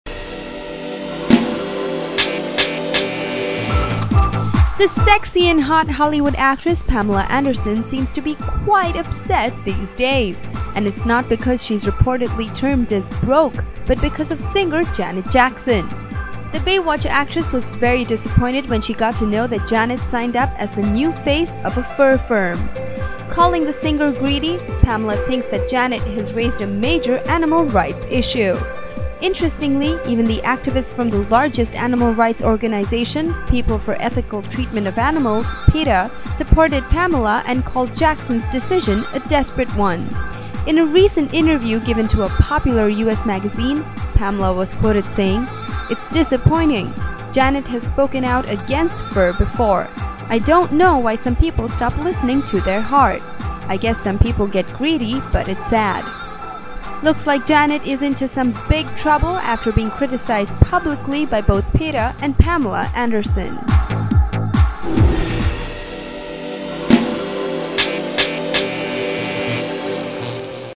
Pamela & Janet Jackson CATFIGHT!
The sexy and hot Hollywood actress Pamela Anderson seems to be quite upset these days.And its not because she was reportedly termed as a broke, but because of singer Janet Jackson.